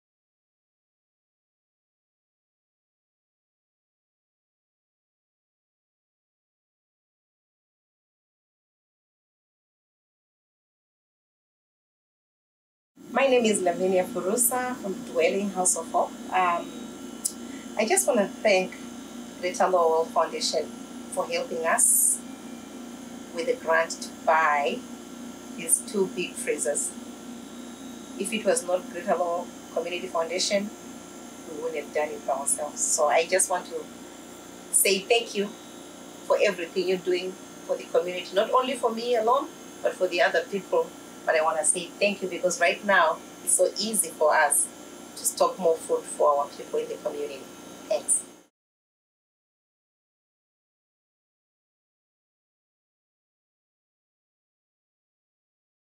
GLCF Grant in Action: Dwelling House of Hope (Lowell)
We love to see a GLCFgrant in action. We had the pleasure of visiting Dwelling House of Hope, Inc. in Lowell this week to learn more about their work and see their shiny new commercial freezers that were purchased with a 2019 Help Alleviate Hunger and Homelessness in Massachusetts grant. This generous one-time capital grant was made possible from an anonymous donor that partnered with the GLCF to distribute the funding to area nonprofits that are battling hunger and homelessness in the community. Thank you Dwelling House of Hope, Inc. for supporting the community.